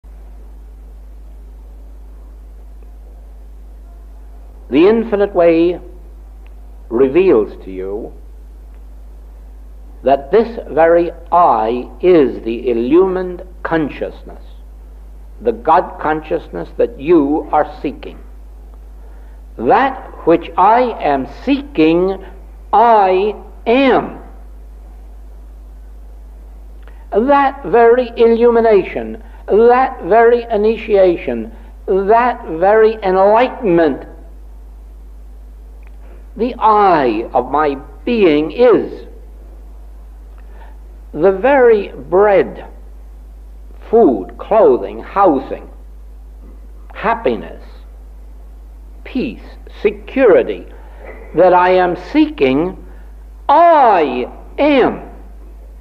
I Already Am
This short video from the "Your Infinite Way" channel emphasizes a core concept from Joel Goldsmith's teachings: the divine presence is already within you.

The speaker highlights that the "eye of your being," your inner consciousness, is the source of everything you seek – illumination, enlightenment, peace, security, and even material needs like food and shelter.

It encourages viewers to recognize that they already are what they are seeking, emphasizing the idea that God consciousness is not separate from you, but is the very essence of your being.

A short piece of audio from Tape532B  from some of his best classwork in London "The Two Major Facets of The Infinite Way"